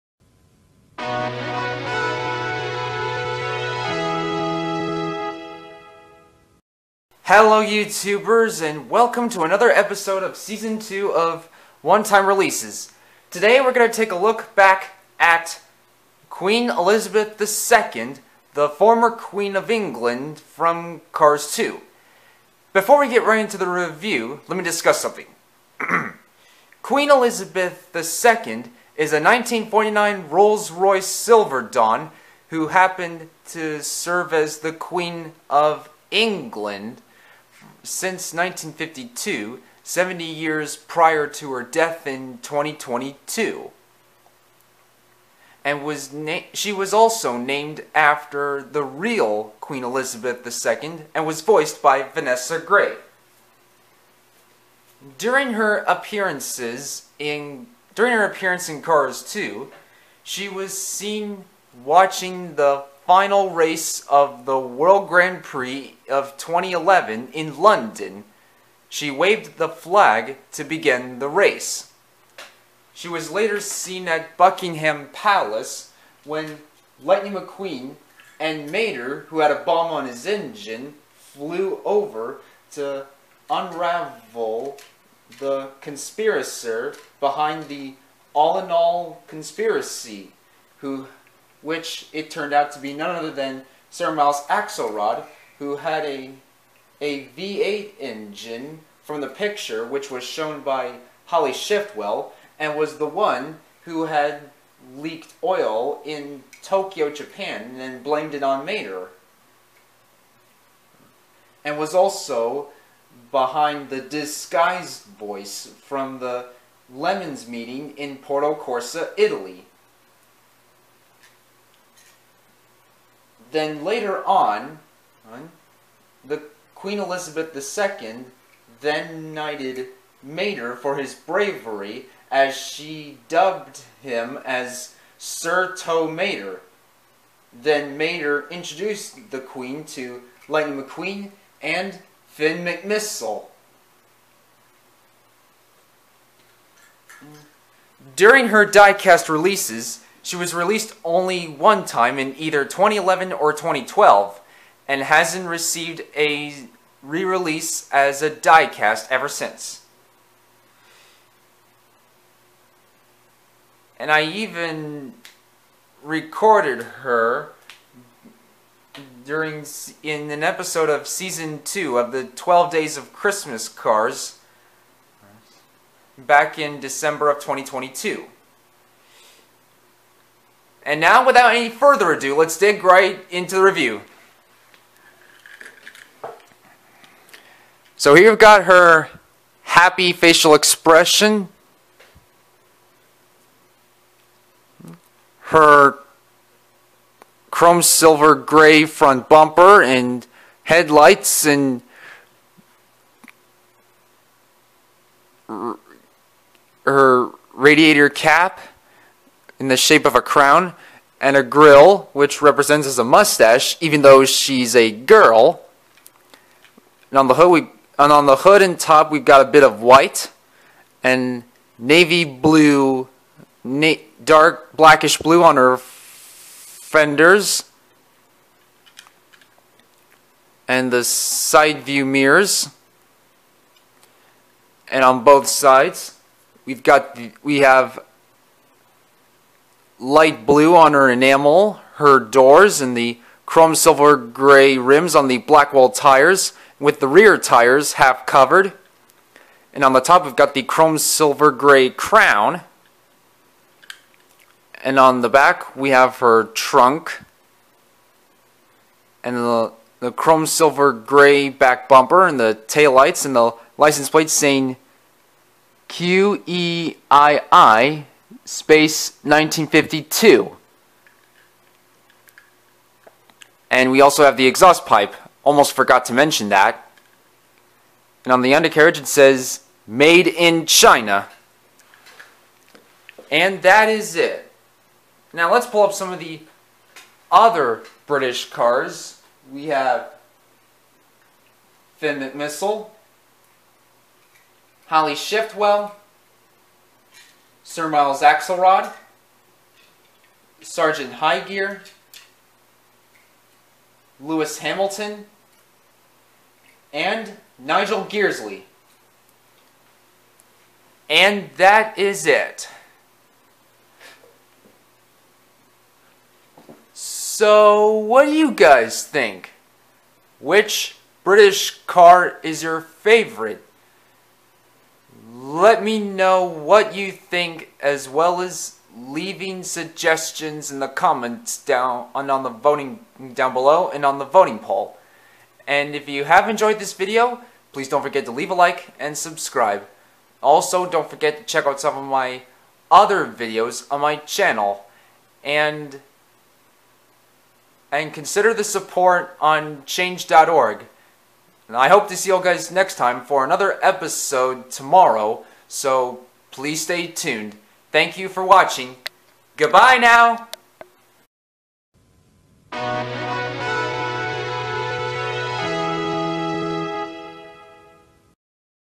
One time releases season 2 episode 8 Queen Elizabeth II
Hello everybody & today, we're gonna take a look back at Queen Elizabeth II (Former Queen of England) from the Cars 2. Consider the support on change.org! Other than that, let me know what you think as well as leaving suggestions in the comments section below!
1949 Silver dawn is a trademark of Rolls Royce.